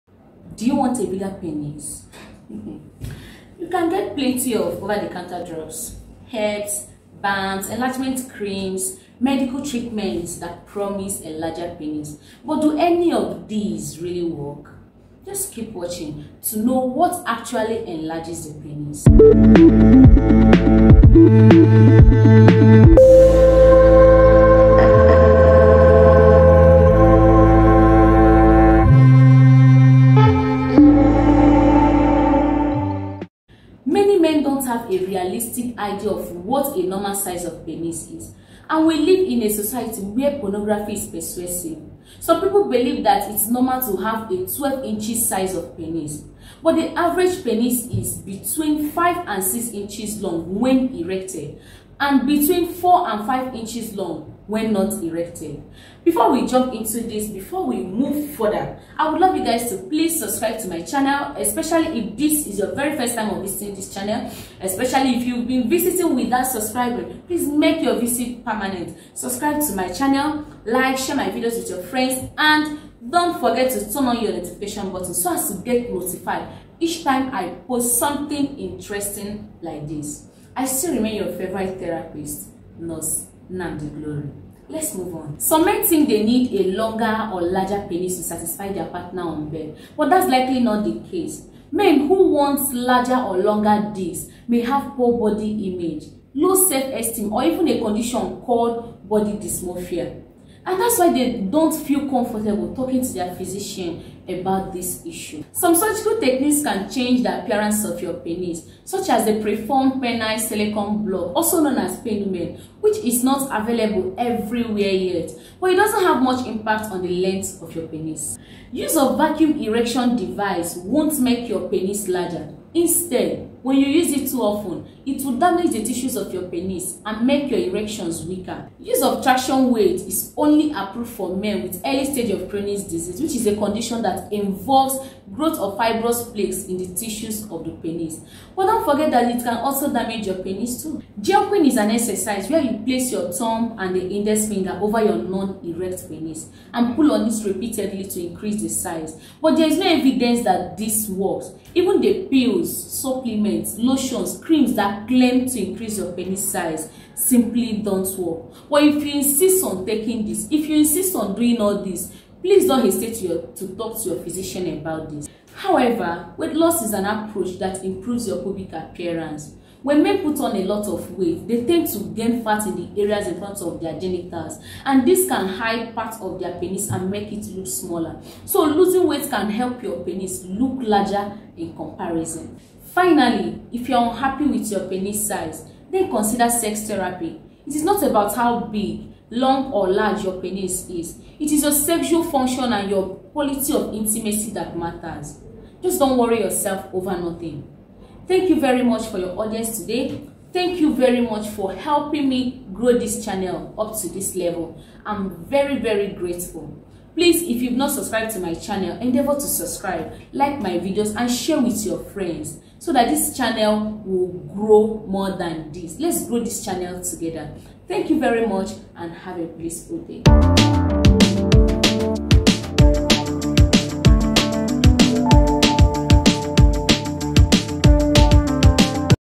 HOW TO ENLARGE YOUR PEN*S BY NURSE GLORY
The average size of a man's Pen*s is 5-6 inches when erected and 4-5 inches when flaccid, so disregard the false societal belief that it should be 12inches. But if you really want to enlarge your pen*s, this video will be of great help.
thanks 😊